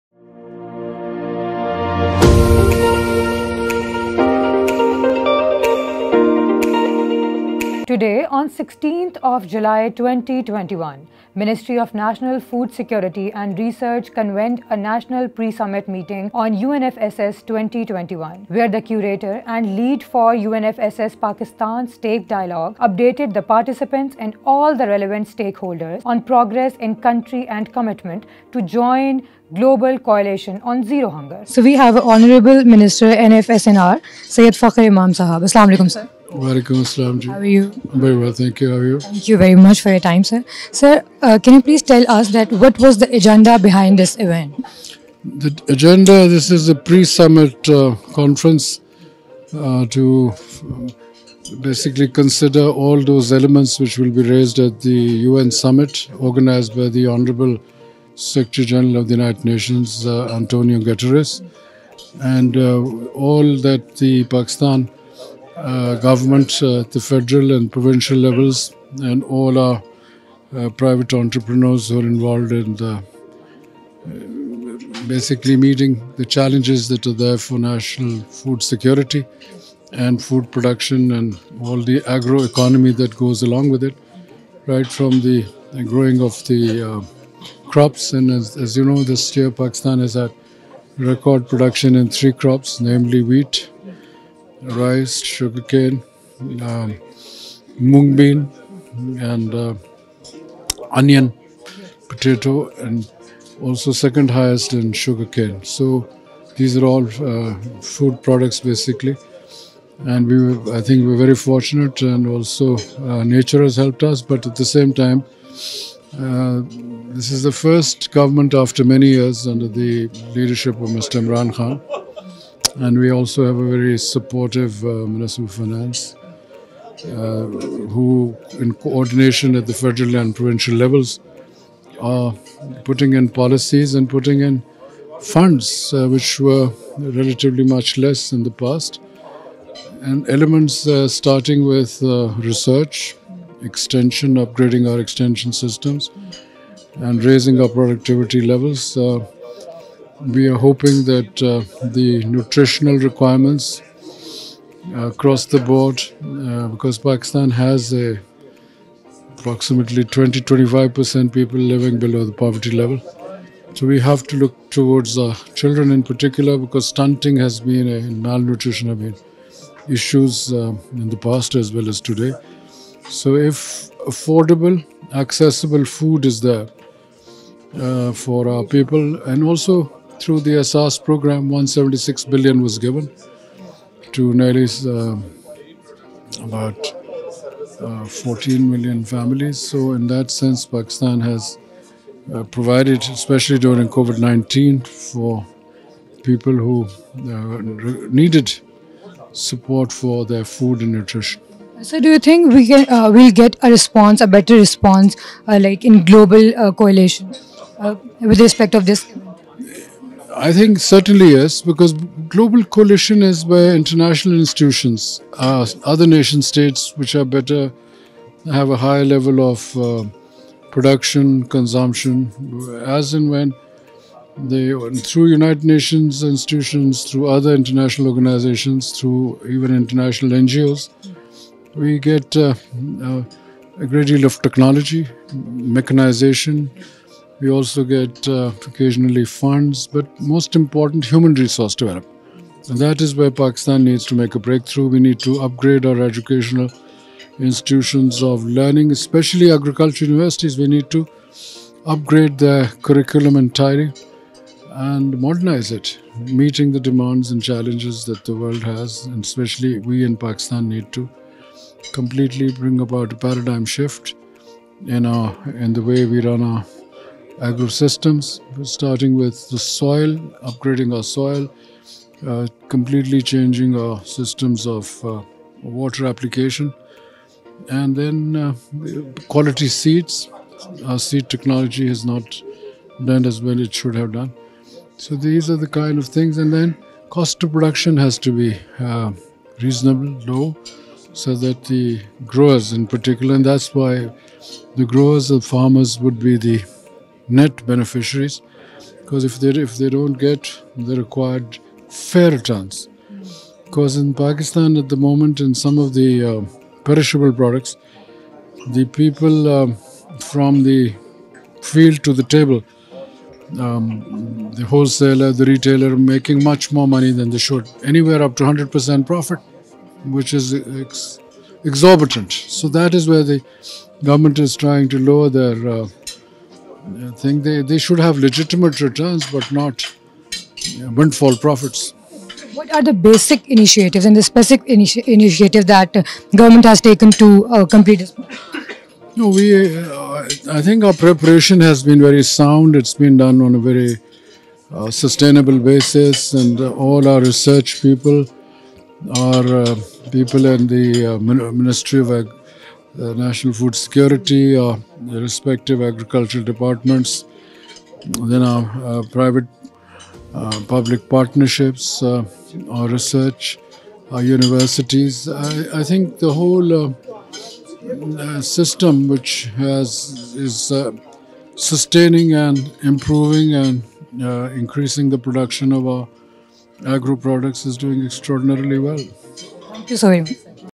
Interview of Syed Fakhar Imam on Pre-Summit UNFSS Meeting On " Pakistan Food System Transformation "
Interview of Syed Fakhar Imam Federal Minister ( Ministry of National Food Security & Research ) on Pre-Summit UNFSS Meeting On " Pakistan Food System Transformation " || GAIN Pakistan